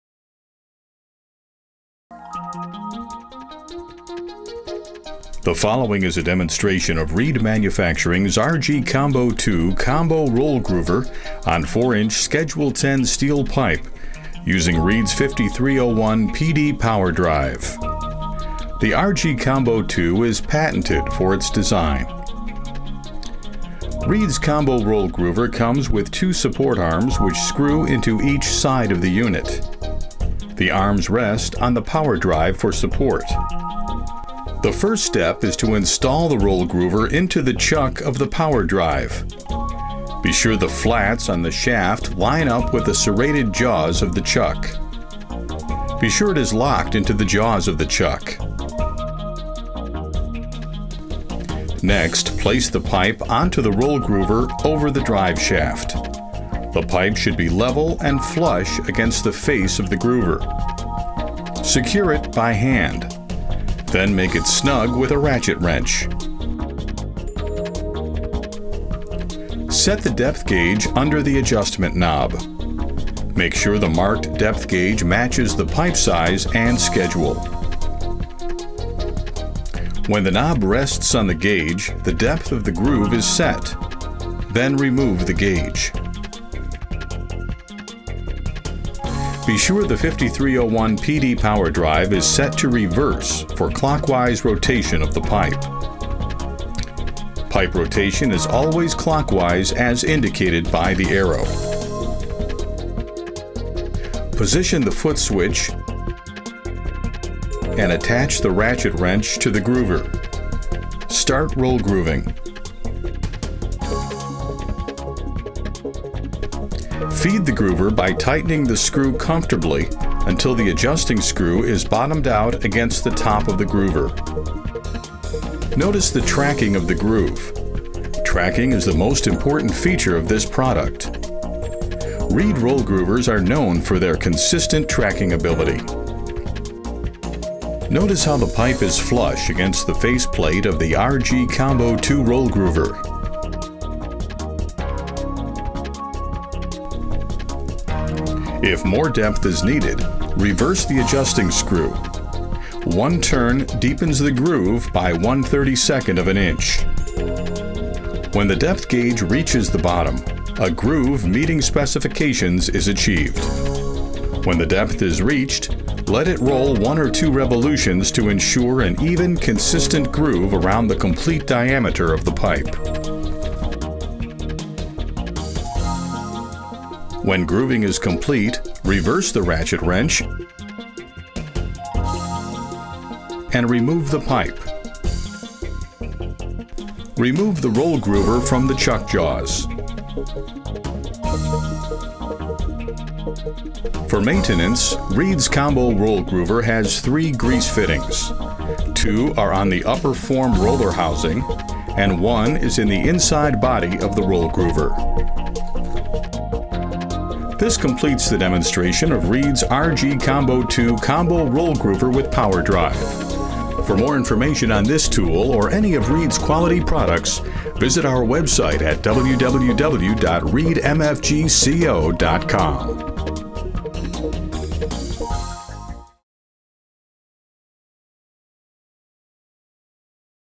Roll Groovers - Grooving with Power Drive Demo - Reed Manufacturing
REED’s Manual or Power Pipe Groover is a 2-in-1 service tools that yield cost savings of time and money by allowing a complete change in minutes from power machine operation to manual work or back.

REED’s Combo Roll Grooving Machine is operated either manually or under power. RGCOMBO2 forms standard rolled grooves in steel, stainless steel, and aluminum pipe from 1 1/4" to 6". The formed grooves comply with the specifications required for mechanical coupling systems. The only adjustment required is for the depth of the groove.

To learn more about REED, to browse all products, or find where to buy, visit our

Let’s Connect!